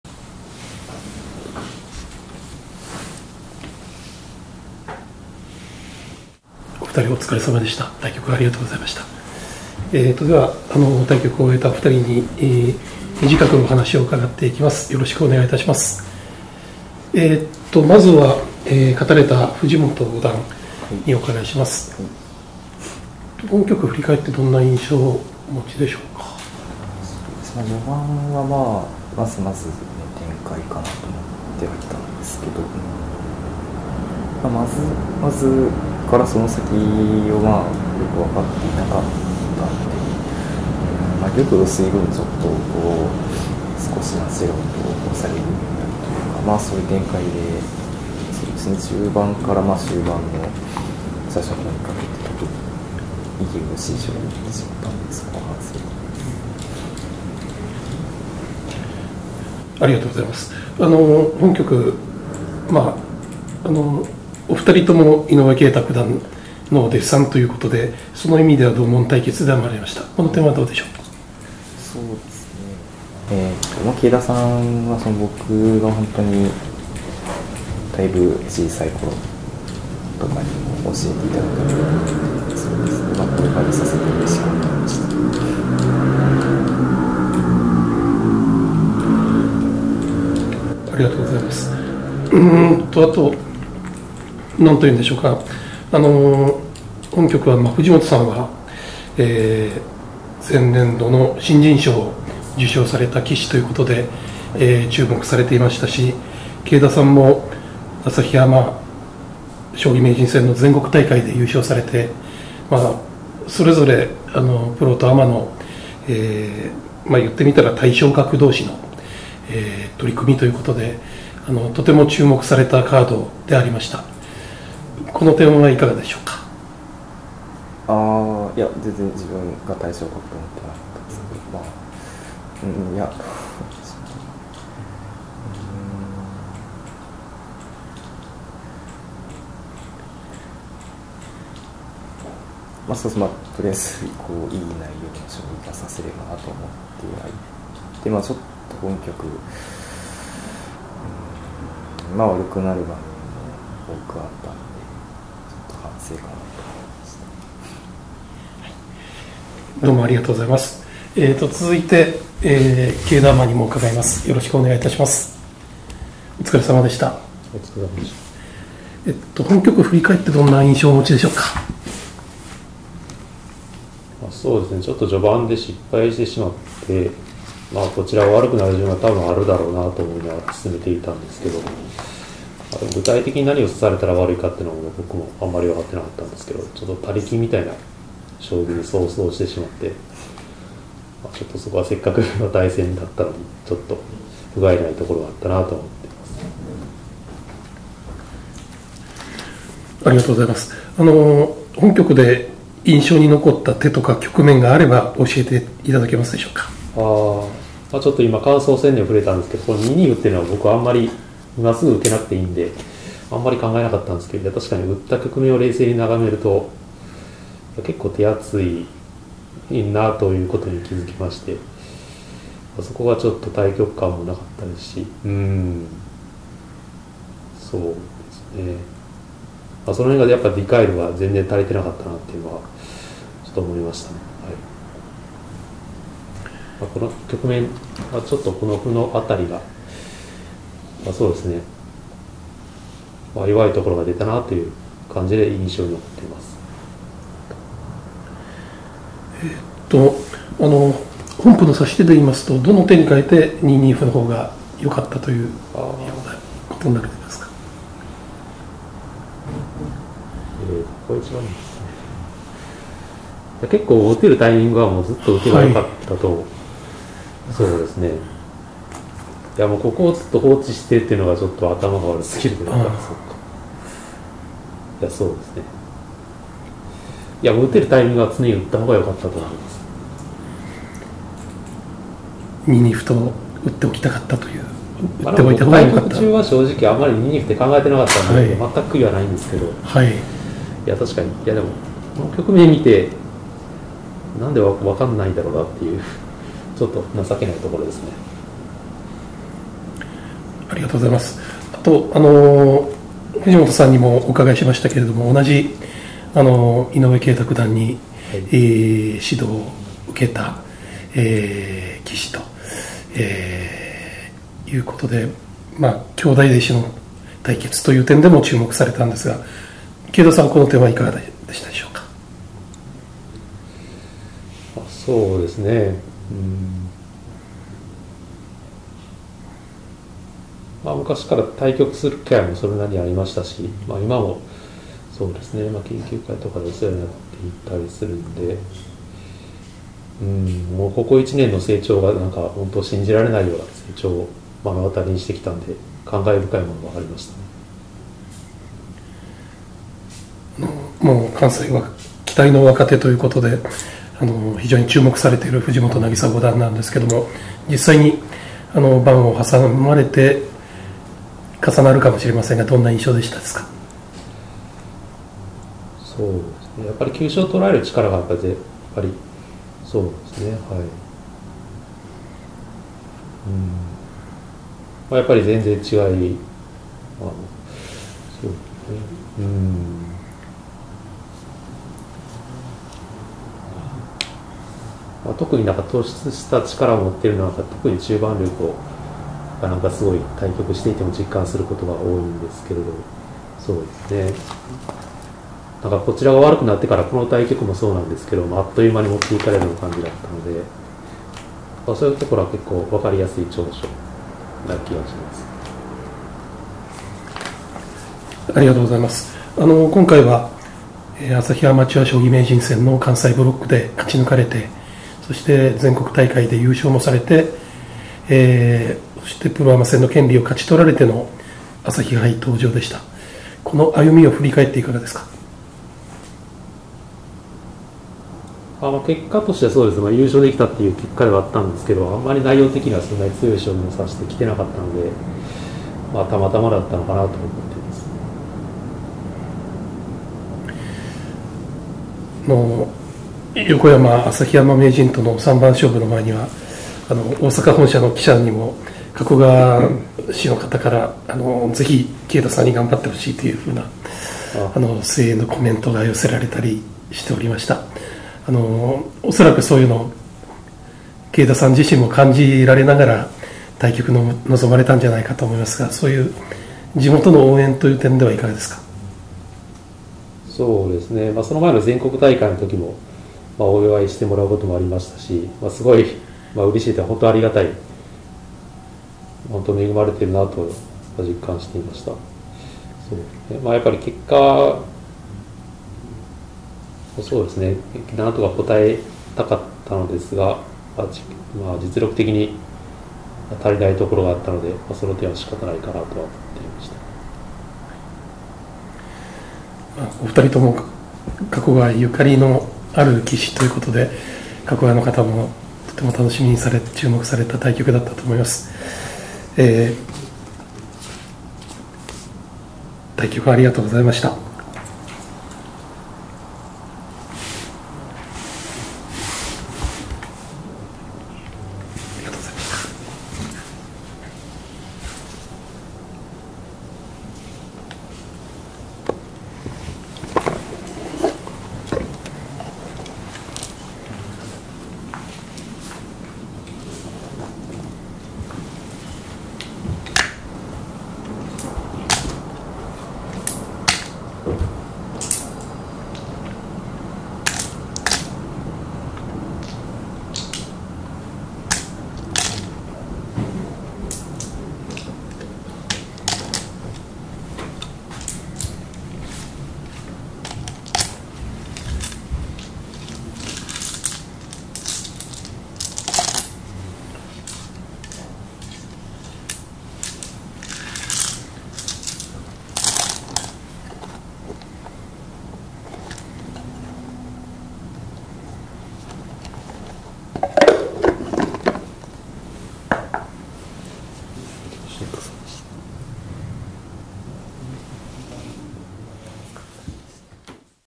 藤本渚五段、同じ井上慶太九段門下の慶田アマと対局「小さい頃に…」【第18回朝日杯将棋オープン戦・1次予選】＝佐藤圭司撮影
第18回朝日杯将棋オープン戦の開幕恒例のプロアマ戦のうち、▲藤本渚五段－△慶田義法アマ戦は好取組。藤本五段は将棋大賞の新人賞も受賞した関西期待の若手で、慶田アマは朝日アマ将棋名人戦全国大会の優勝者。さらに、２人とも井上慶太九段を師匠とし、奨励会で修行を積んだ兄弟弟子の間柄でもあります。結果は藤本五段が相居飛車の将棋を制し、兄弟子に成長ぶりを示しました。